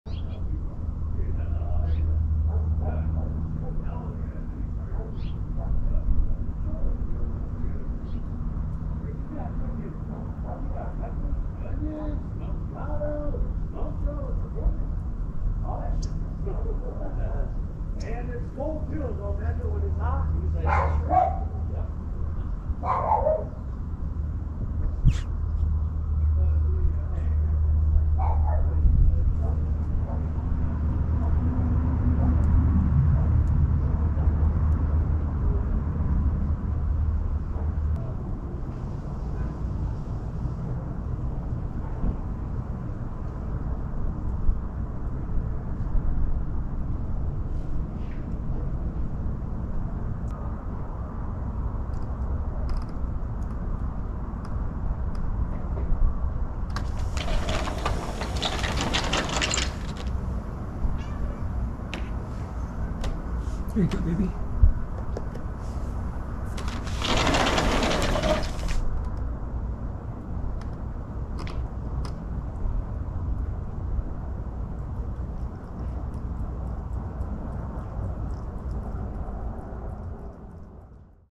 Little Panther Coming For Dinner
The 1 year old fur baby I'm sponsoring coming for dinner